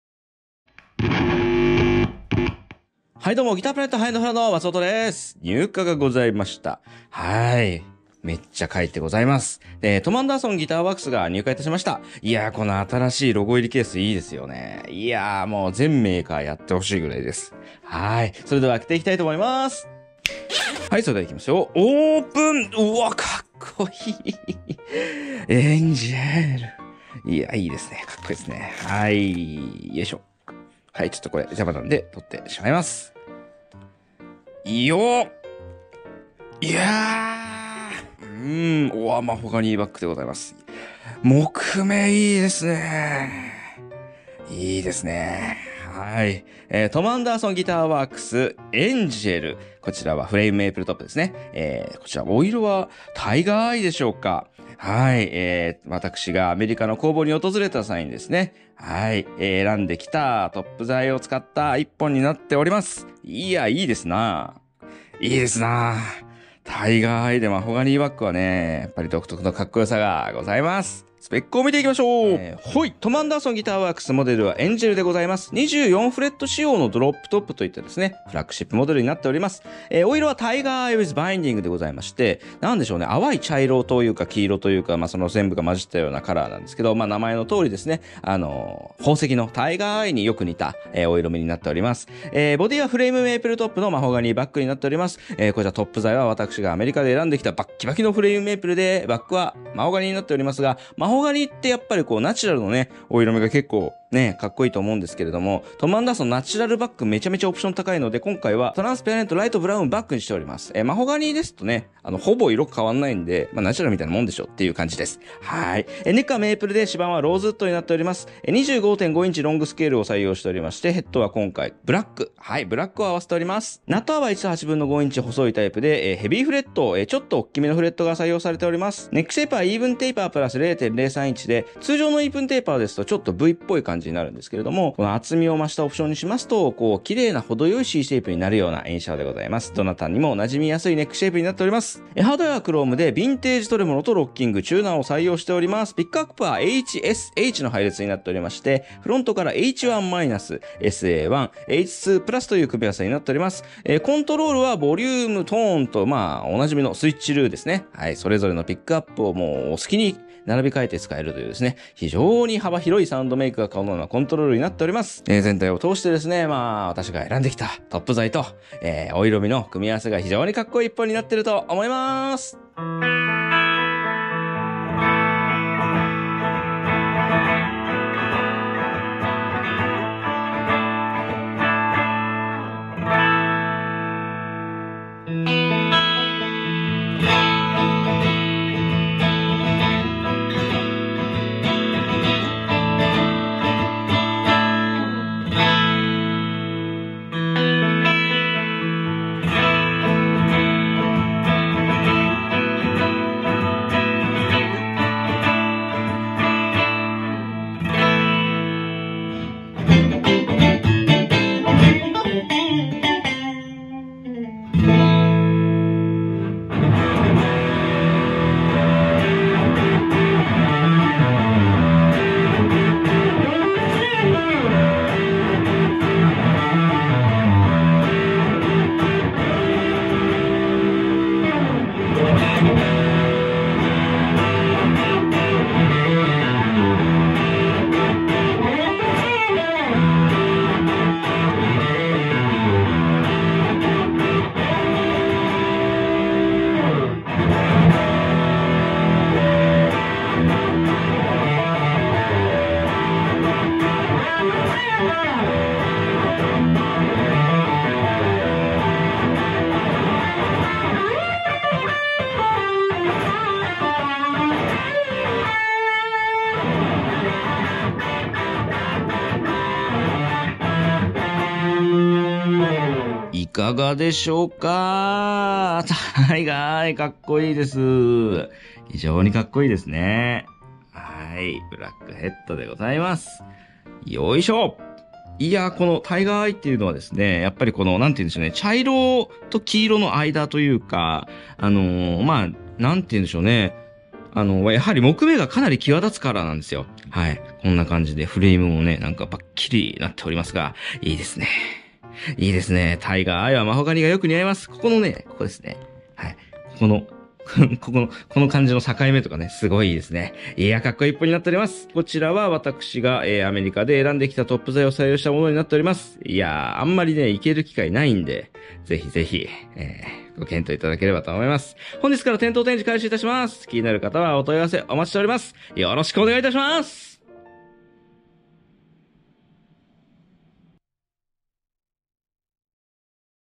開封の儀！Tom Anderson Angel Selected FMT Tigereye with BInding【商品紹介@Guitar Planet】
Tom Anderson Angel Selected FMT Tigereye with BIndingのご紹介です。

松本が工房にて現地選定した極上のトップ材を用いた当店カスタムオーダー品！
虎杢が映えるタイガーアイフィニッシュが印象的な個体です！